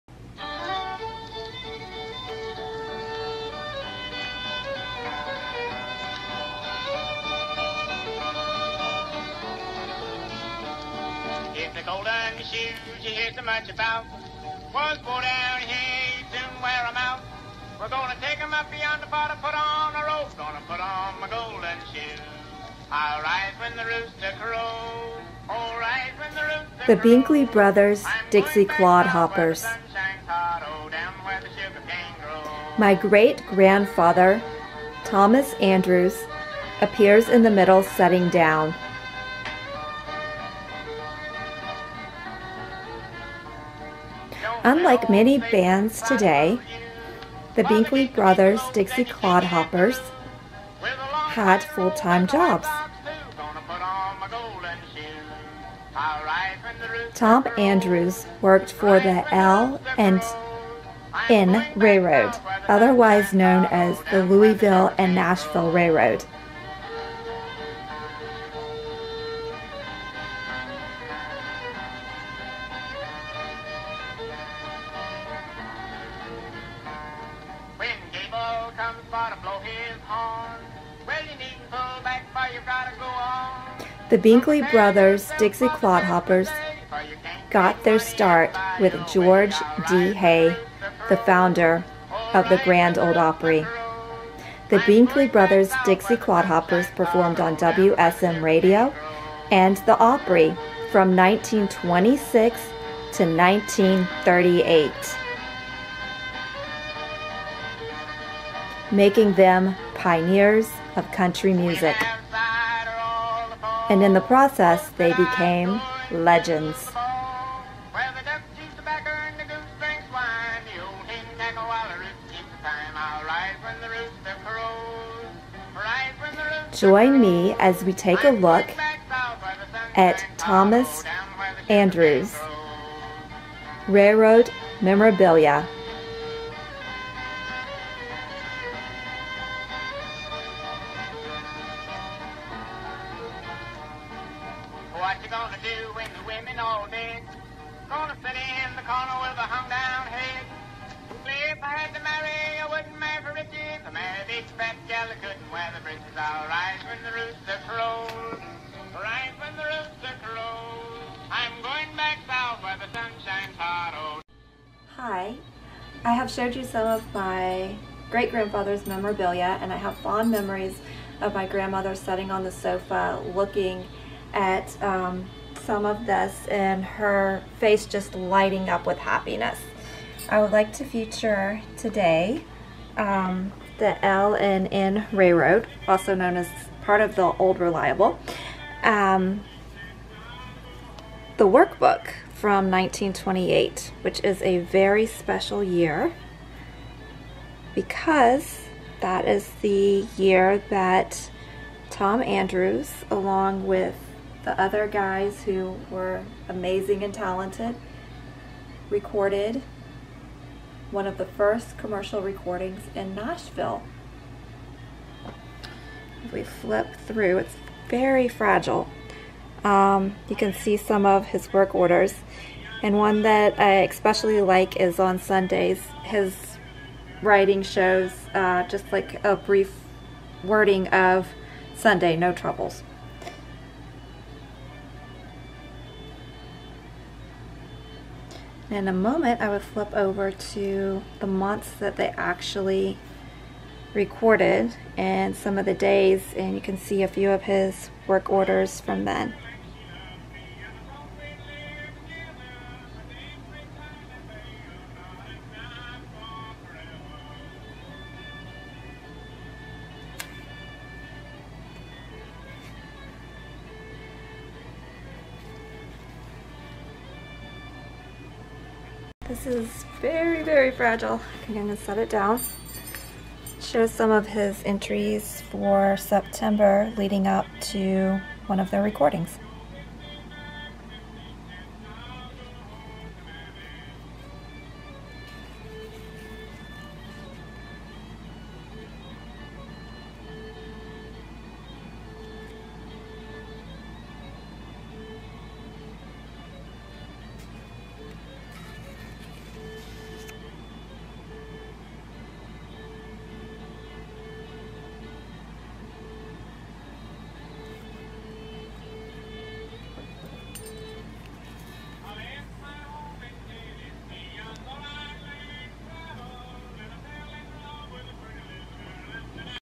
Tom Andrews Binkley Brothers' Dixie Clodhoppers L&N Railroad memorabilia.
Tom Andrews's great-granddaughter Bella, presents his Louisville and Nashville Railroad memorabilia.

Tom Andrews was a member of Binkley Brothers' Dixie Clodhoppers and was one of the first to preform on "Grand Ole Opry" on WSM.

While not playing in the Binkley Brothers' Dixie Clodhoppers, Tom Andrews worked for the Louisville and Nashville Railroad.